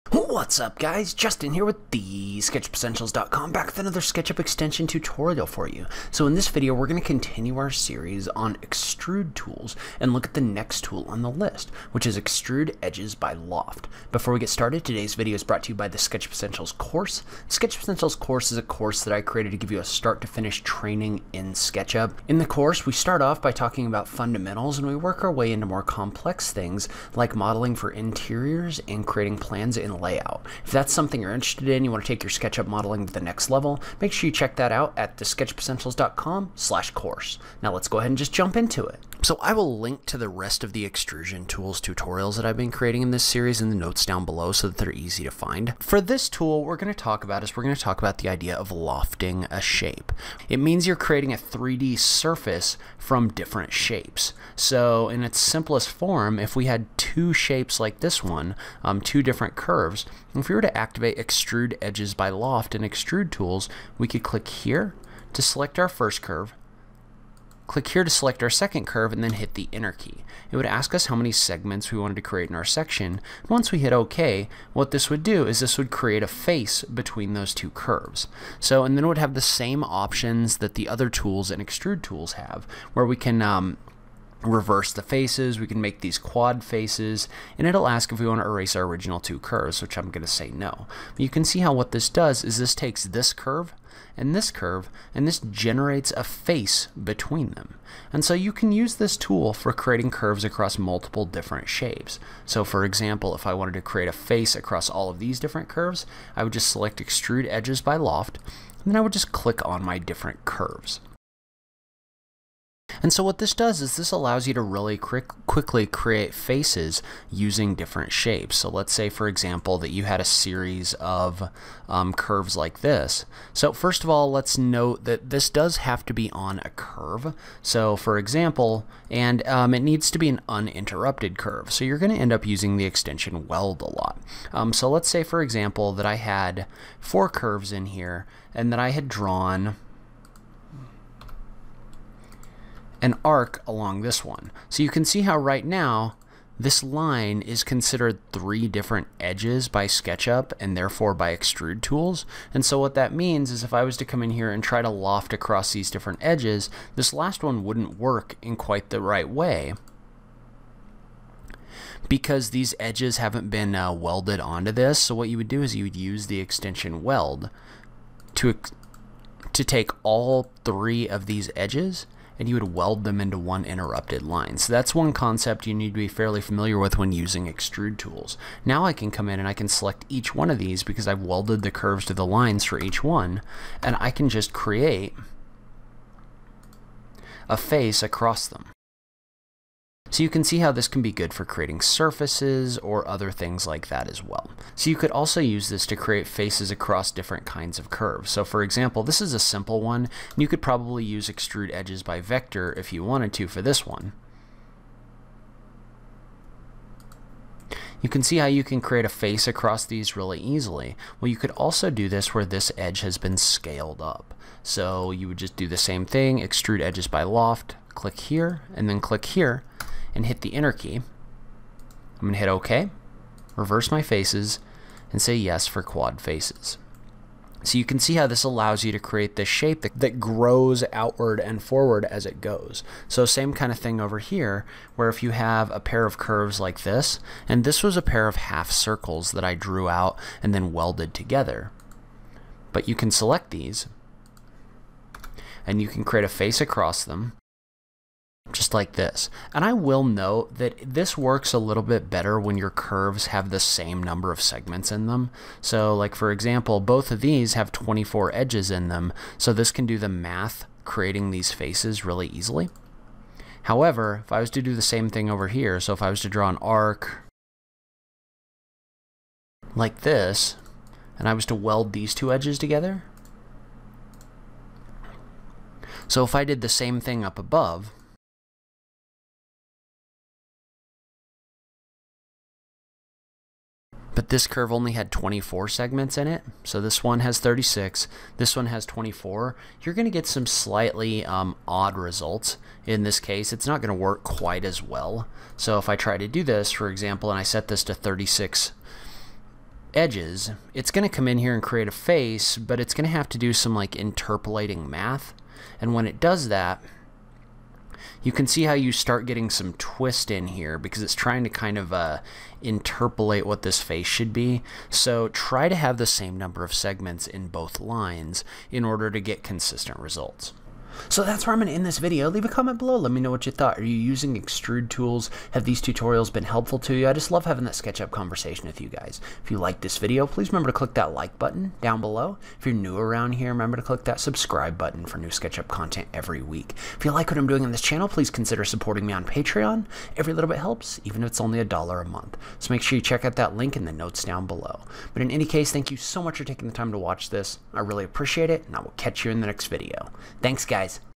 SKETCHUP EXTRUDE TOOLS TUTORIALS (EP3) - Extrude Edges by Loft for SketchUp
In this video, learn to use the tool extrude edges by loft within the SketchUp extension Extrude Tools to create a face using multiple edges! This can be helpful for creating complex structures and much more!